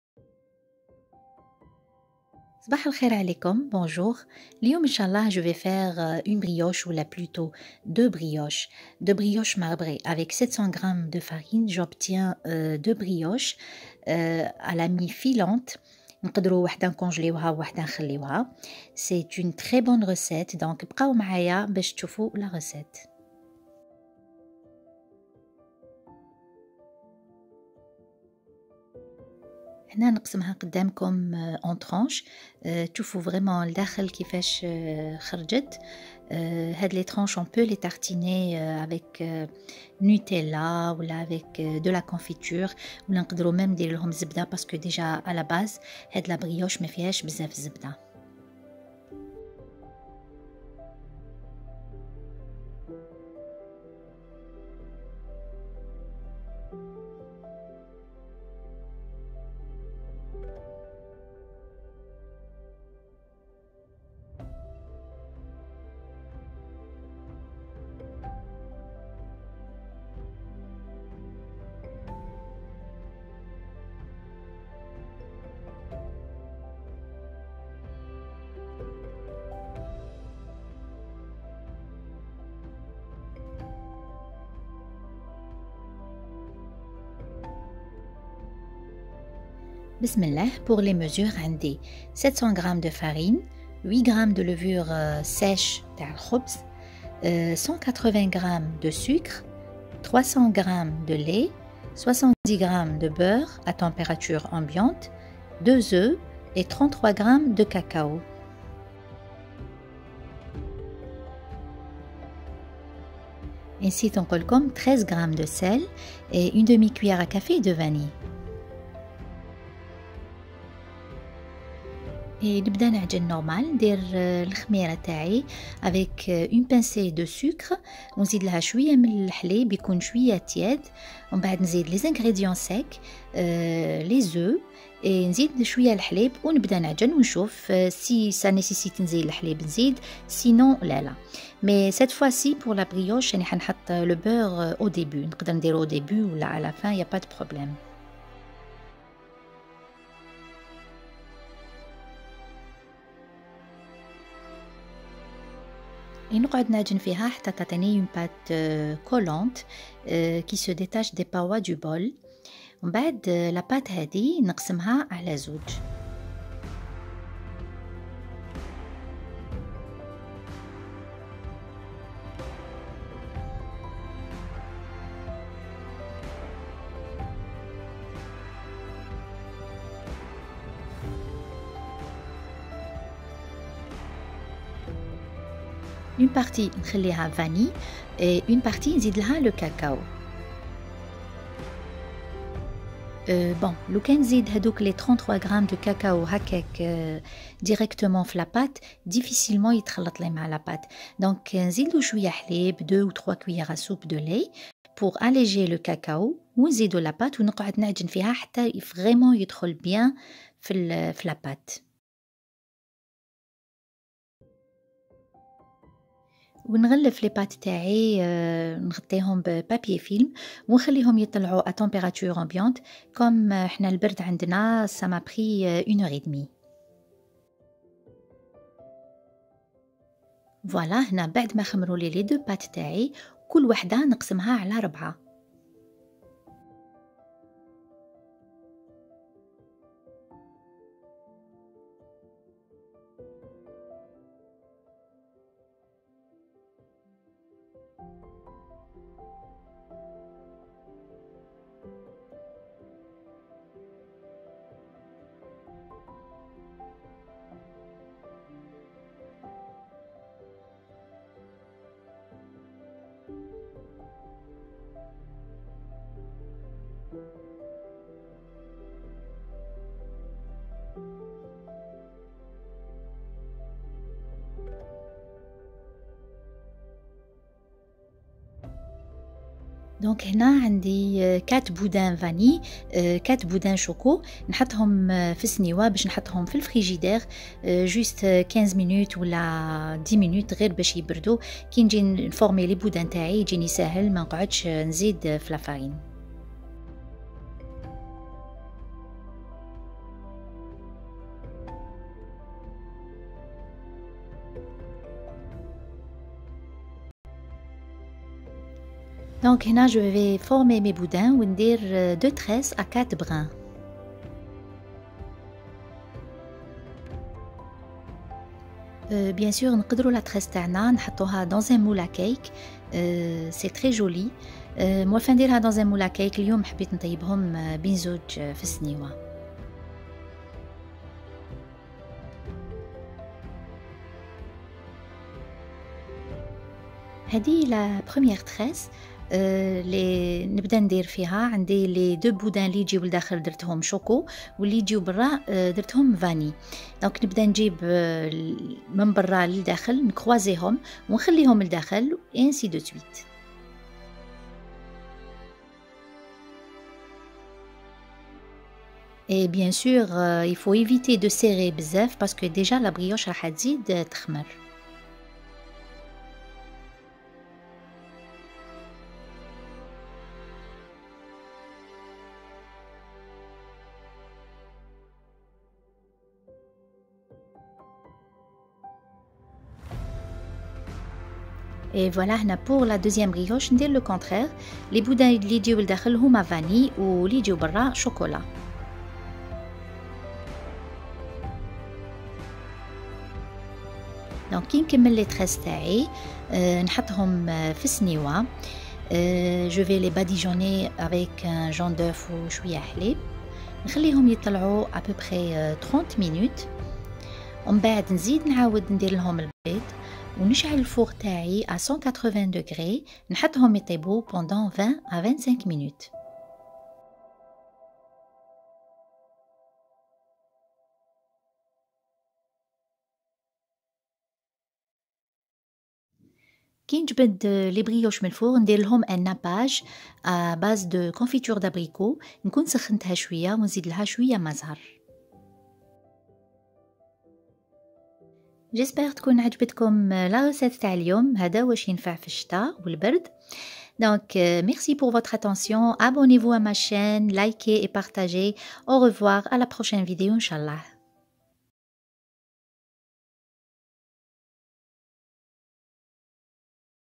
BRIOCHE MARBRÉE
Ingrédients :
700g de farine
8 g de levure sèche
1/2 cuillère à café de vanille
180 g de sucre
300 g de lait
70 g de beurre à température ambiante
2 œufs
33 g de cacao mélangé avec 2 à 3 cuillères à soupe de lait
13 g de sel
1 jaune d'œuf pour la dorure
Un peu de confiture d'abricot
Eau de fleur d'oranger